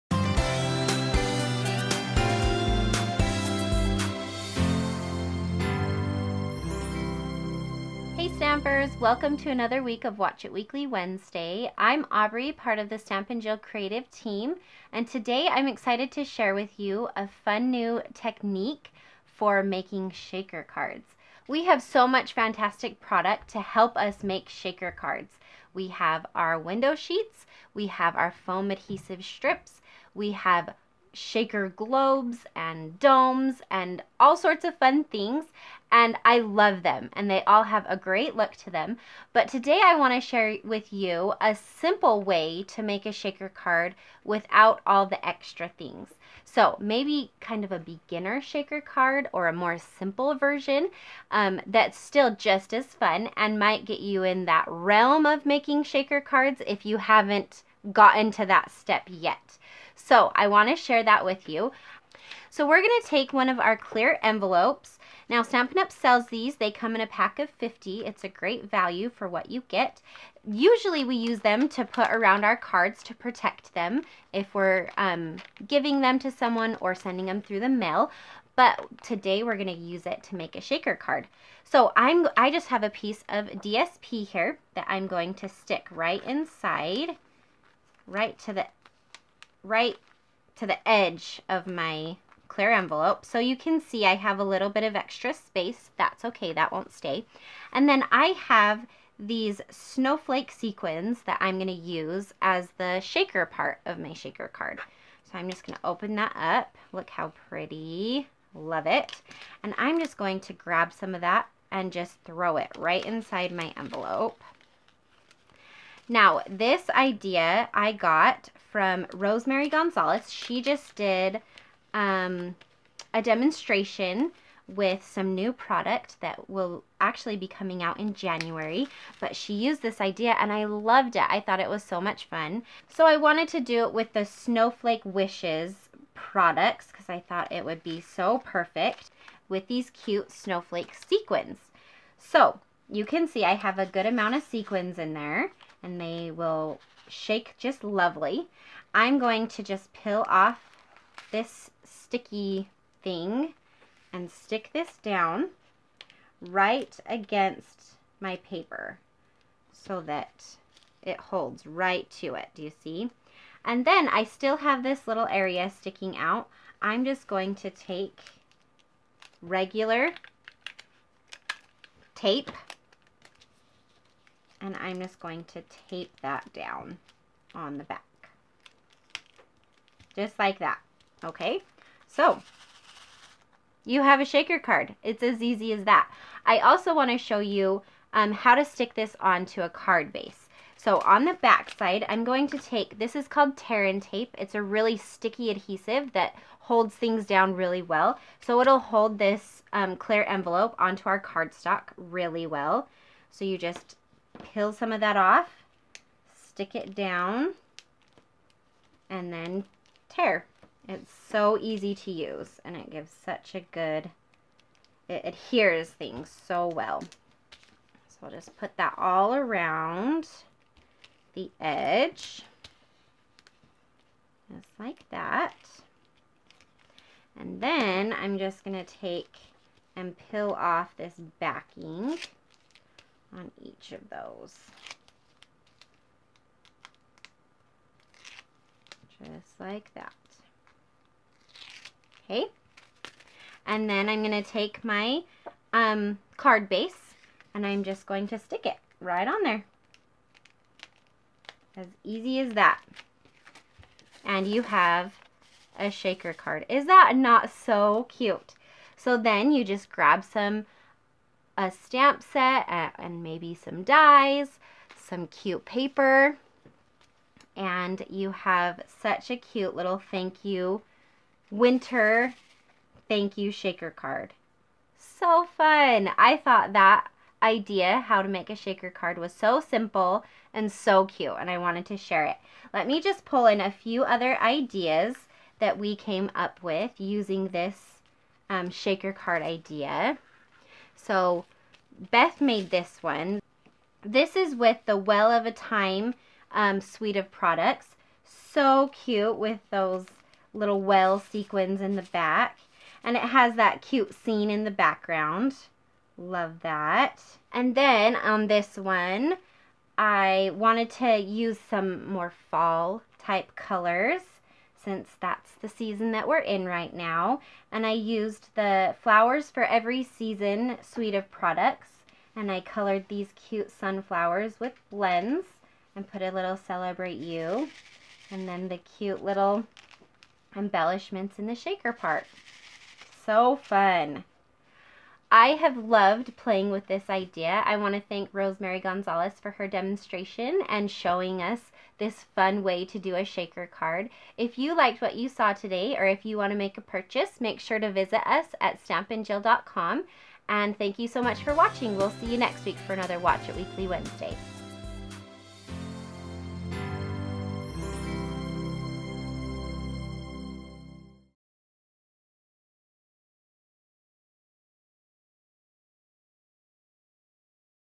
Stamping Jill - Simple Technique On How To Make A Shaker Card
Stampin' Up! Demonstrator Aubrey demonstrates an easy and simple technique on how to make a shaker card. We are always about new techniques and fun way to create cards. Check out how easy this technique is.